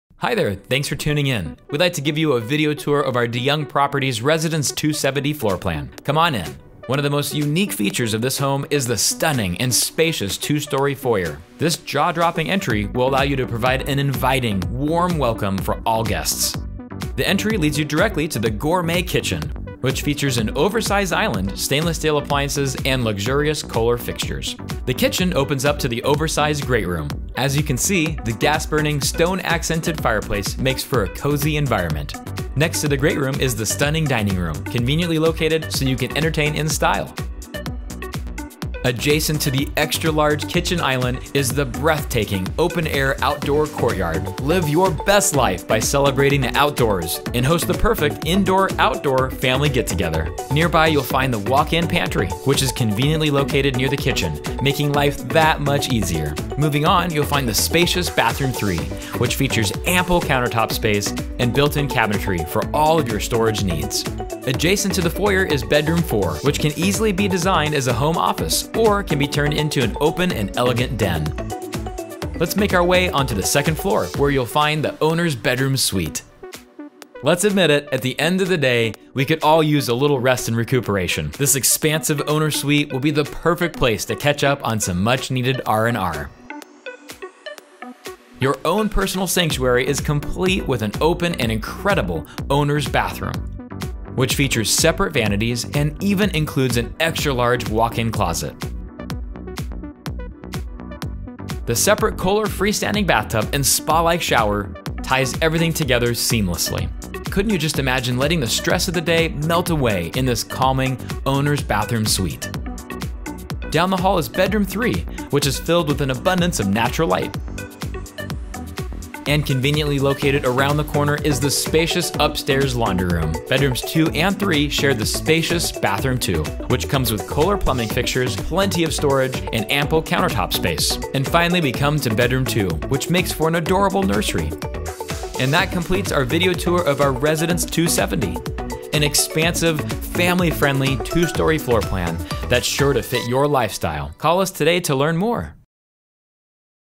Residence 270 Video Tour - A New Home by De Young Properties
This expansive two-story home features a grand entrance and up to 5 bedrooms, 3 bathrooms, and 3 car tandem garage, as well as a gourmet kitchen open to the open-air outdoor courtyard, great room & formal dining room! De Young SmartHome Icon Series floorplans incorporate leading edge building science technology and include Advanced Framed Walls. To personalize your De Young home to fit your lifestyle, De Young offers a wide variety of selections to Flex Your Floorplan including a Multi-Generational design, a first floor bedroom suite, a first floor bedroom suite or den for even more living space and 4 car tandem garage! Enjoy the spacious 10’ ceilings you will find throughout the first floor and 9’ ceilings on the second floor. This home is available in 3 different elevations to fit your taste and style.